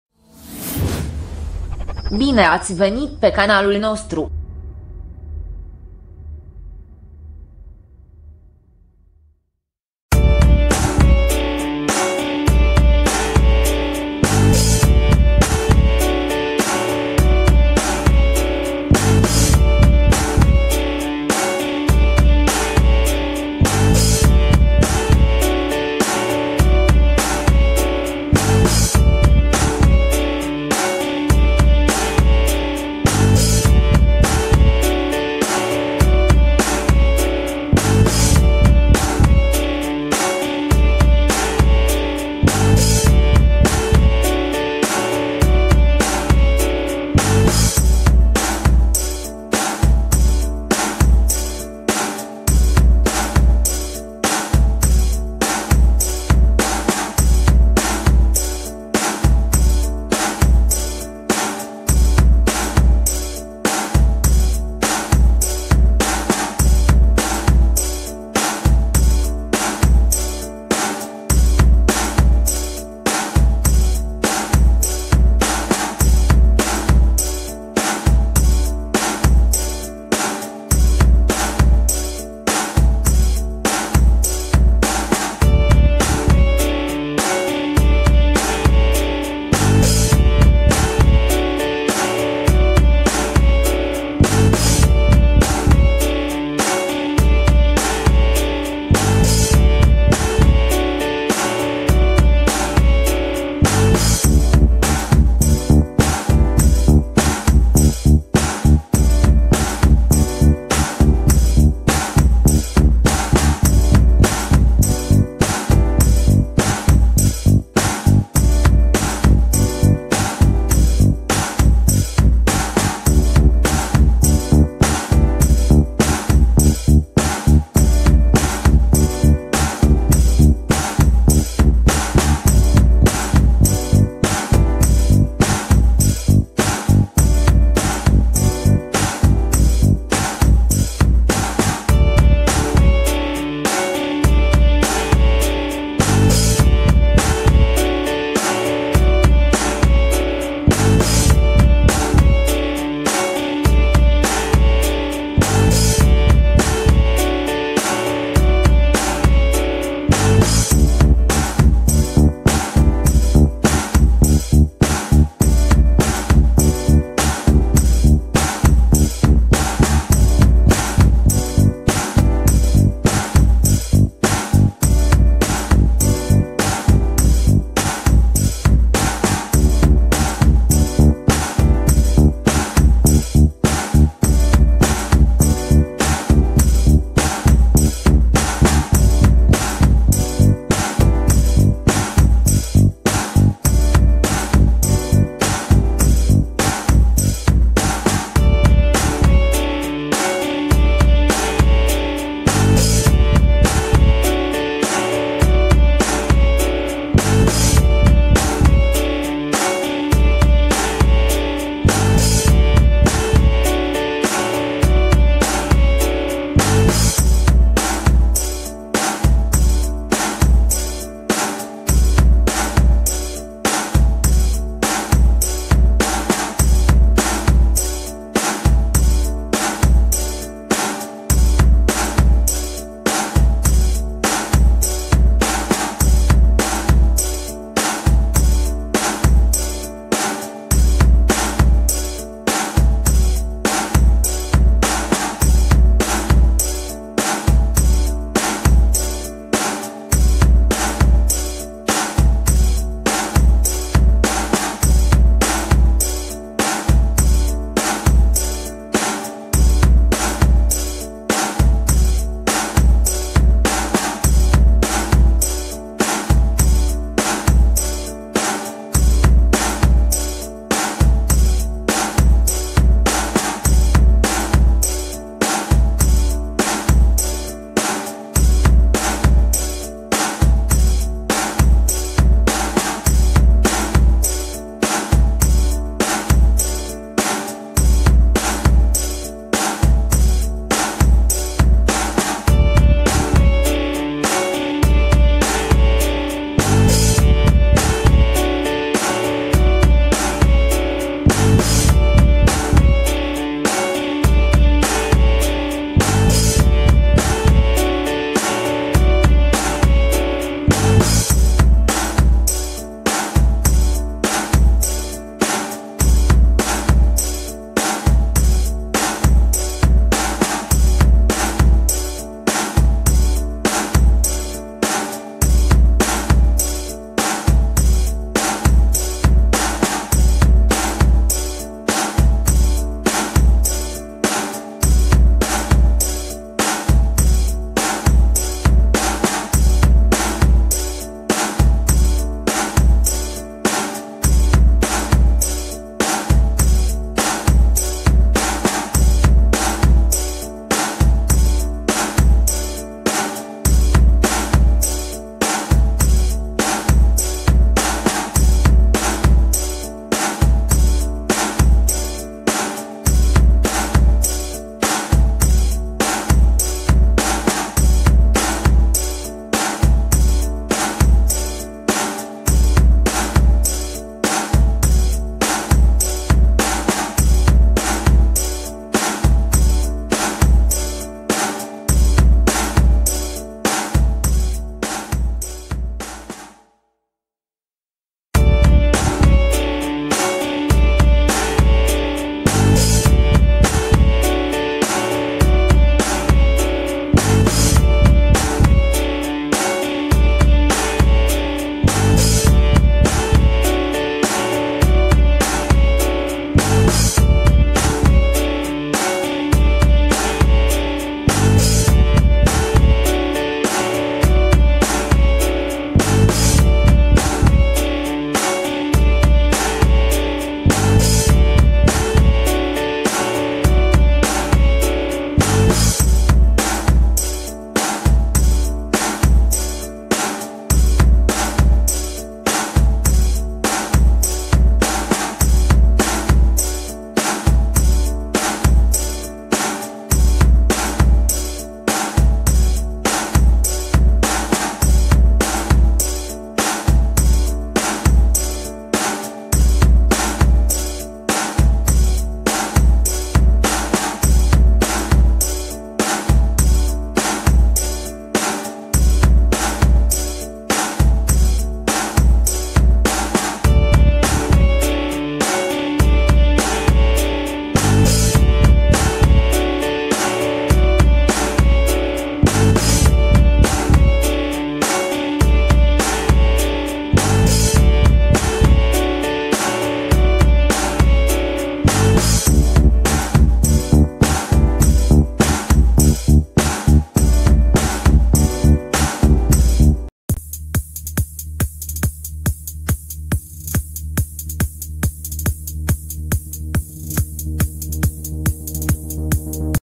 Ziua VIP-urilor la ICCJ. Dosarele lui Dragnea, Tăriceanu și Oprea. Ce spune liderul PSD
UPDATE 16,00: "Am înţeles acuzaţiile ce mi au fost aduse din rechizitoriu. Le resping categoric deoarece mă consider nevinovat", le-a spus judecătorilor preşedintele PSD, la termenul de miercuri al procesului. Liviu Dragnea a explicat că în perioada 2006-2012 a activat în cadrul Consiliului Judeţean Teleorman, ca preşedinte, fiind şi şeful PSD Teleorman, precizând că nu avea nicio relaţie privilegiată cu cele două femei care ar fi fost angajate fictiv la Direcţia de Asistenţă Socială din judeţ. ...